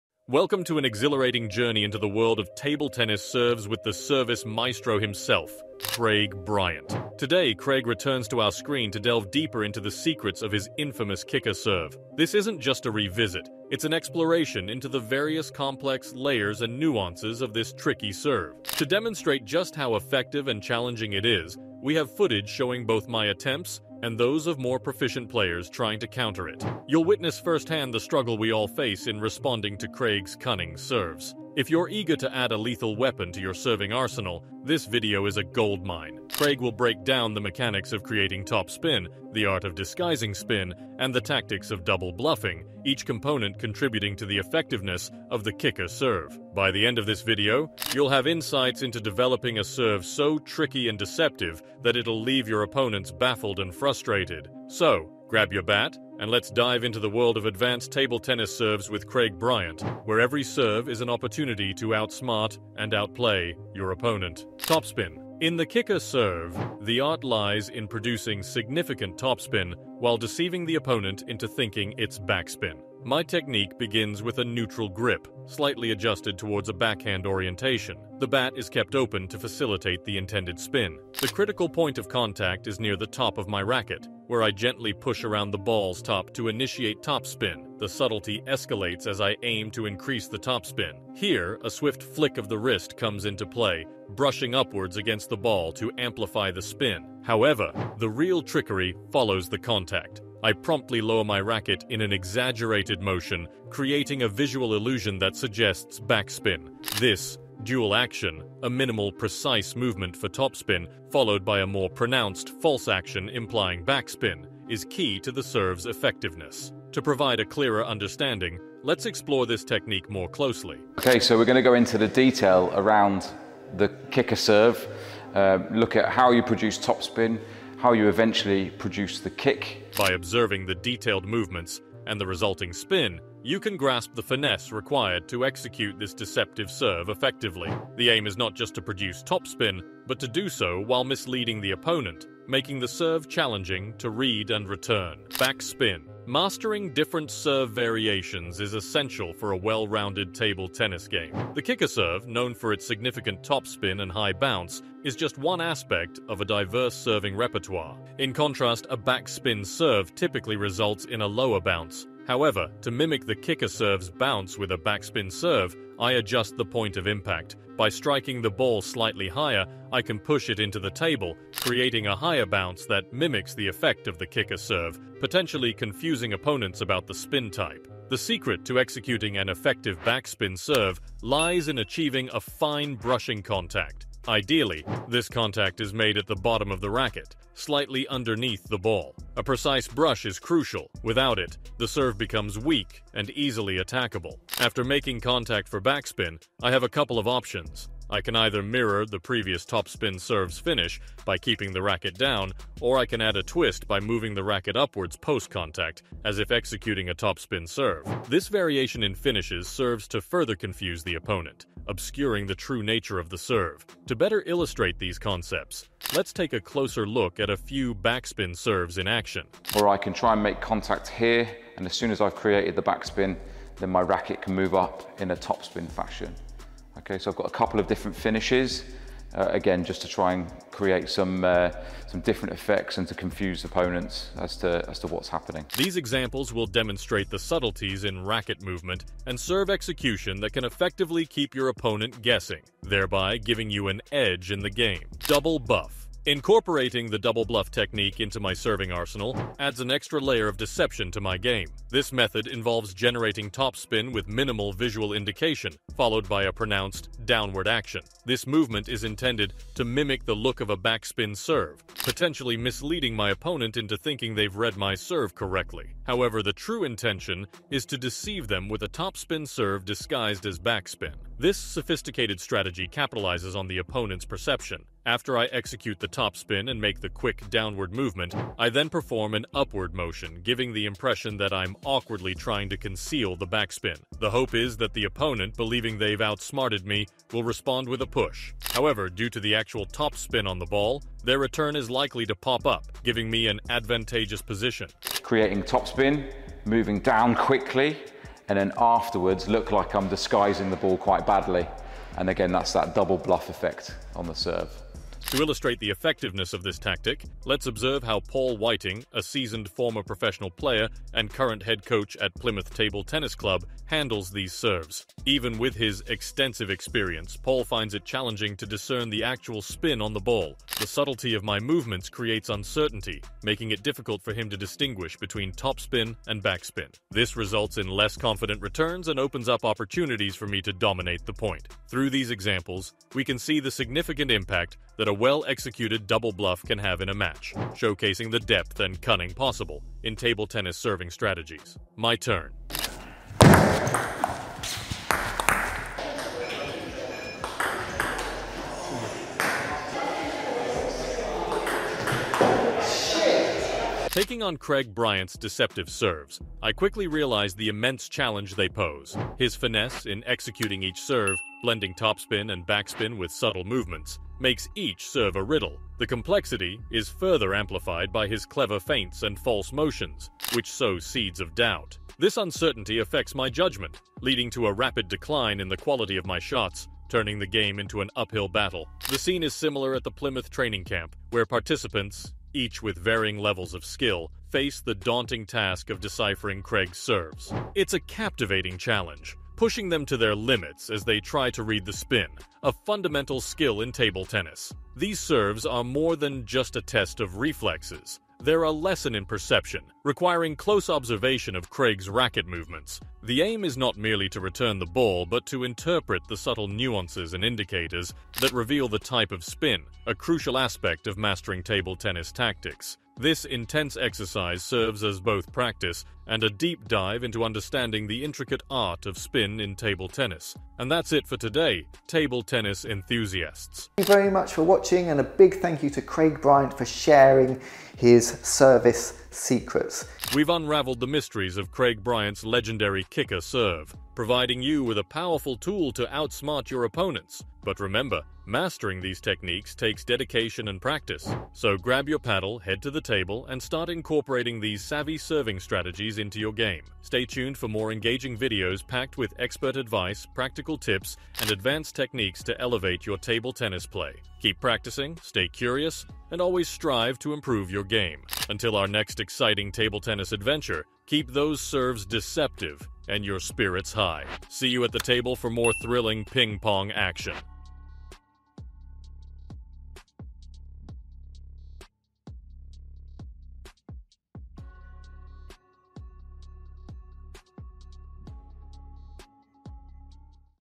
DEADLY Kicker Serve in Ping Pong!
Elevate your game with the DEADLY Kicker Serve!
In this video, we unveil the secrets behind a game-changing serve that will keep your opponents on their toes.
Master the art of imparting spin, add unpredictability to your shots, and watch as your opponents struggle to return the Kicker Serve.

Whether you're a beginner or a seasoned player, this tutorial breaks down the technique step by step, empowering you to take your Ping Pong skills to new heights!